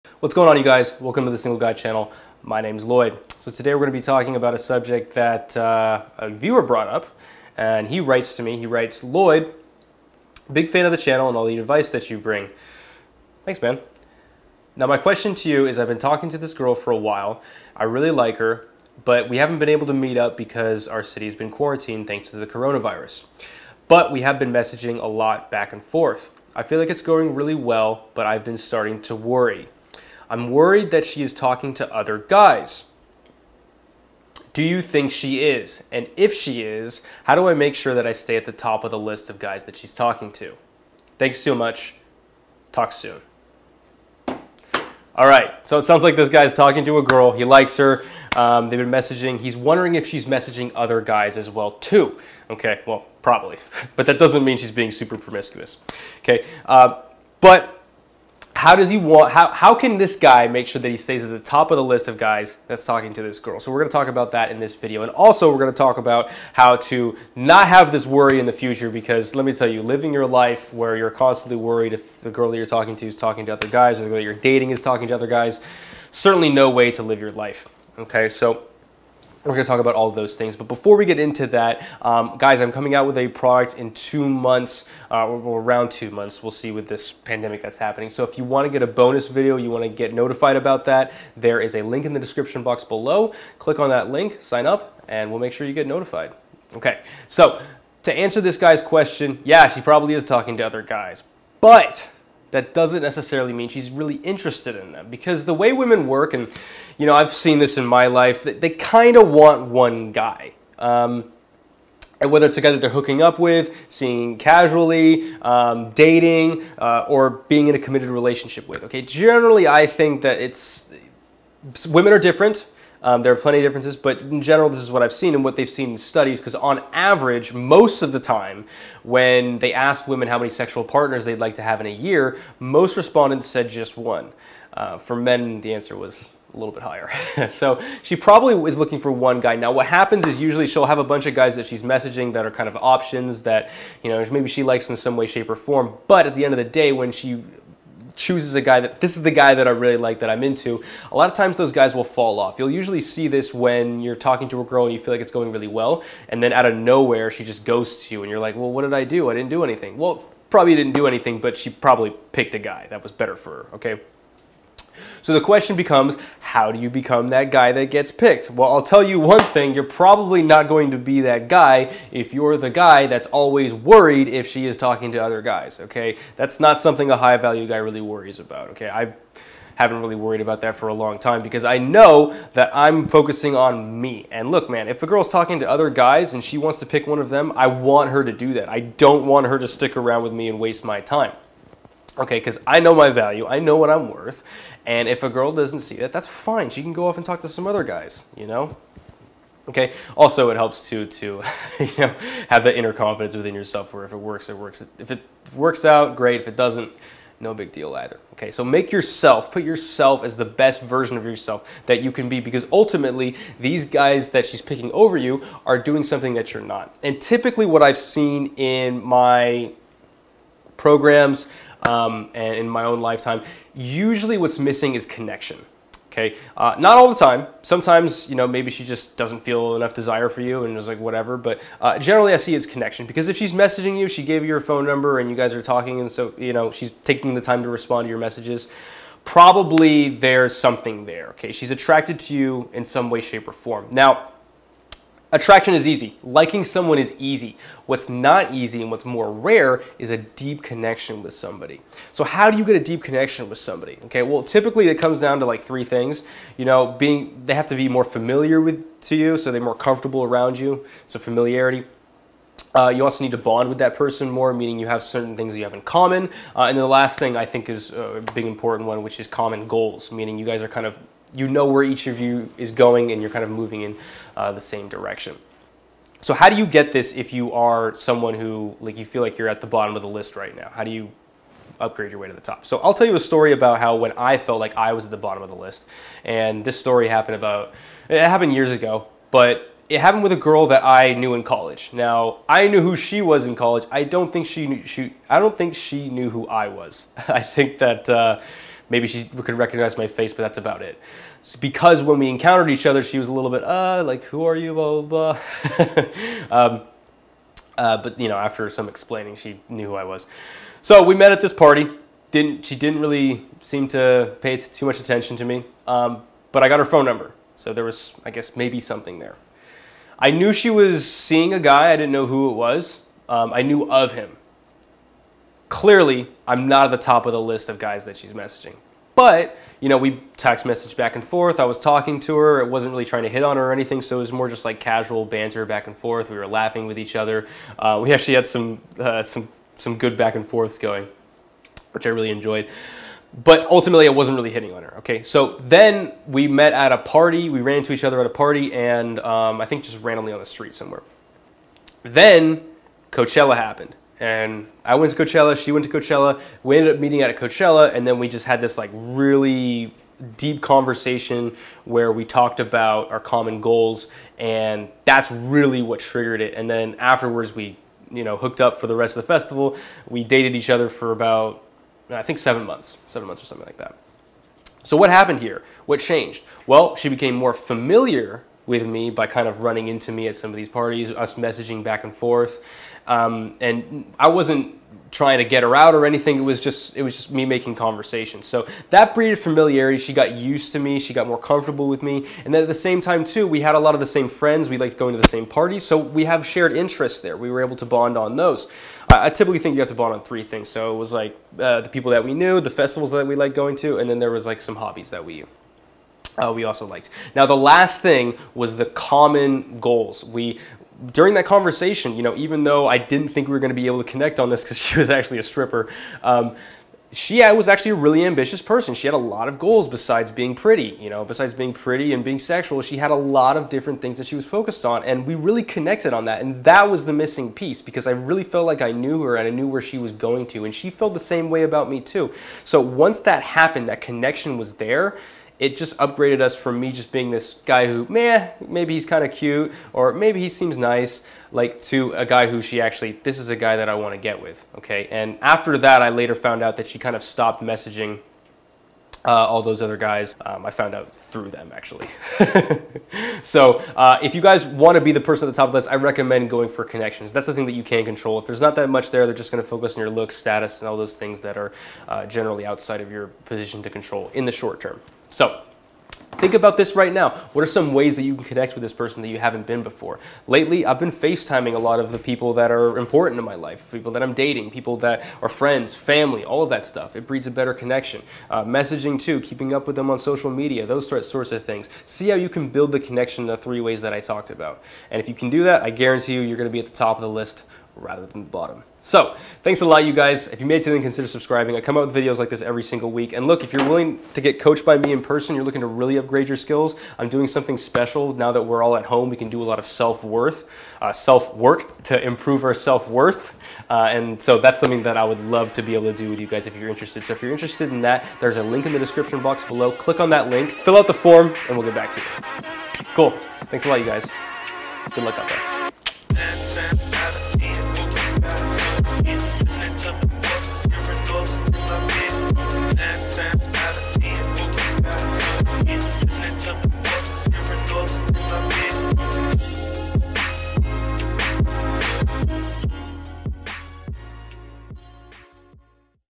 Is she talking to other guys? How to beat your competition
It sucks when you worry if the girl you like is talking to other guys. News flash gents: she probably is. Now that we know this what can we do to increase our chances of eventually being the only guy she's talking to. In this video I talk about exactly that and cover an example of how to do this from my own life.

For access to free trainings and exclusive videos get on my VIP list: